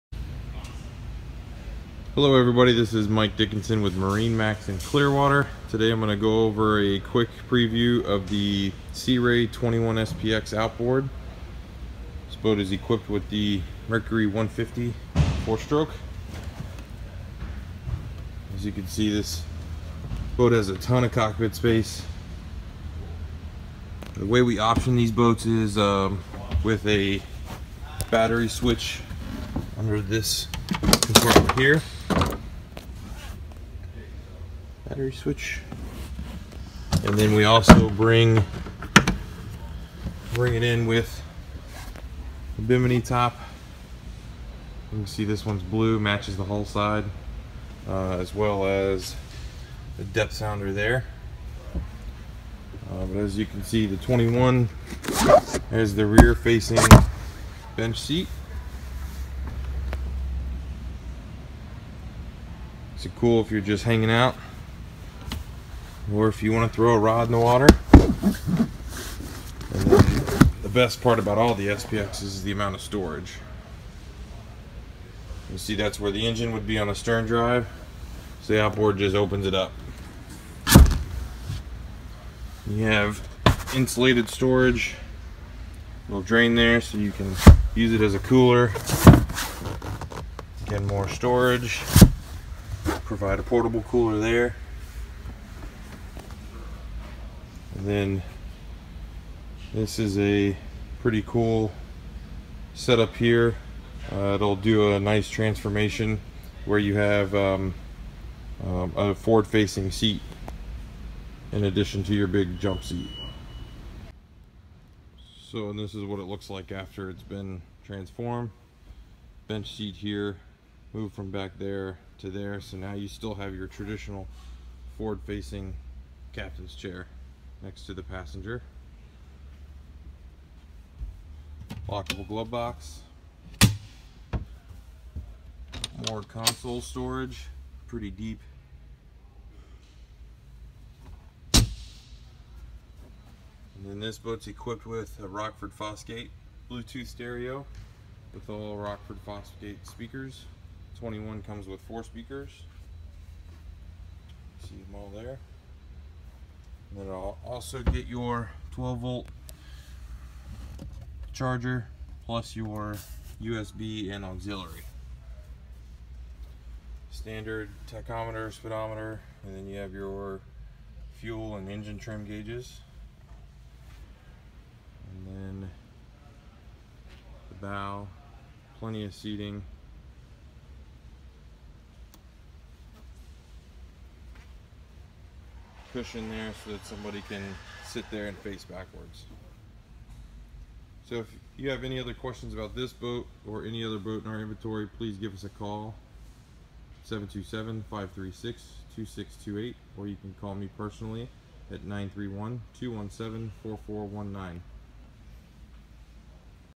2017 Sea Ray 210 SPX Outboard Boat For Sale at MarineMax Clearwater
Outboard enthusiasts will love this adventurous runabout. Our SPX 210 Outboard with 150 hp Mercury EFI outboard offers the maneuvering flexibility and hands-on satisfaction many sport boat lovers are looking for. In addition, an optional "All Sport" Package includes casting chairs, battery charger, livewell and trolling motor. And for further functional customization, check out the optional packages.